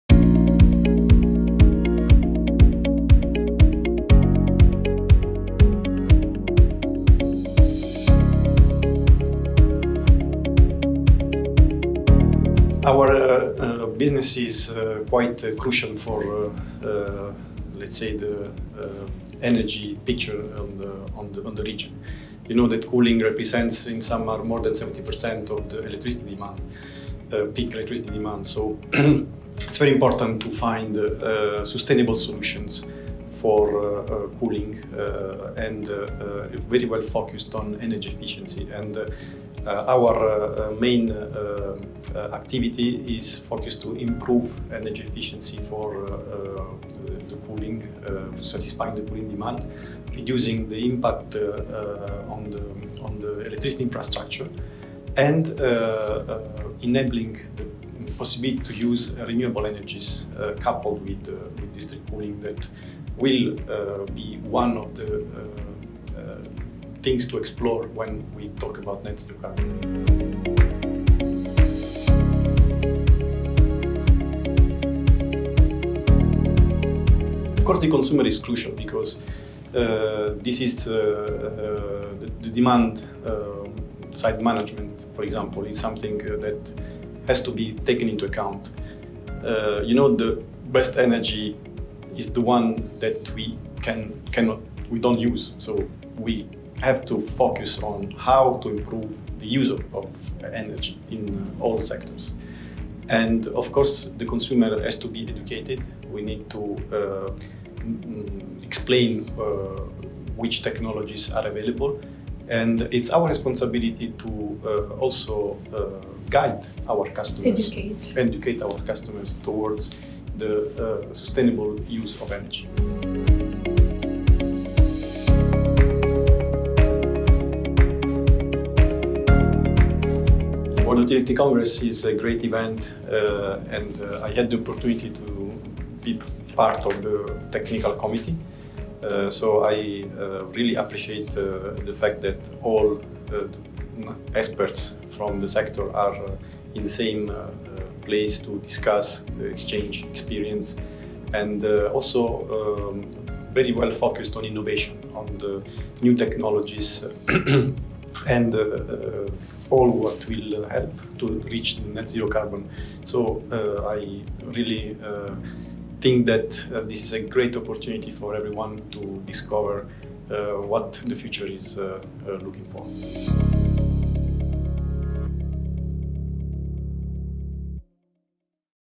Interview with Antonio Di Cecca - COO, Tabreed
Antonio Di Cecca - COO, Tabreed and Executive Committee Member for World Utilities Congress 2023 talks to us about the importance of sustainable solutions and improving energy efficiency for the cooling industry.

World Utilities Congress, hosted by TAQA Group, will take place in-person at the ADNEC (Abu Dhabi National Exhibition Company) in Abu Dhabi from 8-10 May 2023.